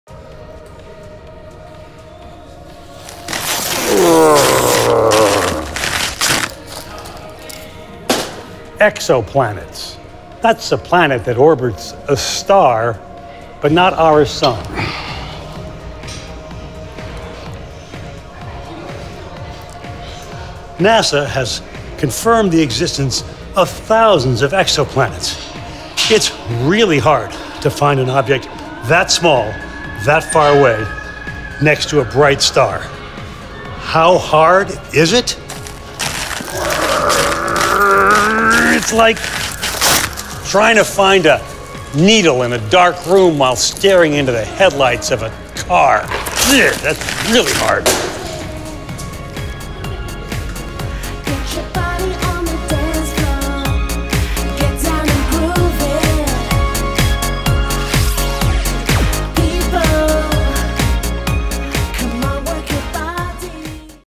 Öö, ekspolaneetta? Bruce Oreck kertoo. Uutiskäsisetteet selkokielellä, osa 2.
Bruce Oreck repii puhelinluetteloita ja kertoo eksoplaneetoista. Uutiskäsitteet selkokielellä, osa 2. Helsingin Sanomien uusi somesarja. 2017.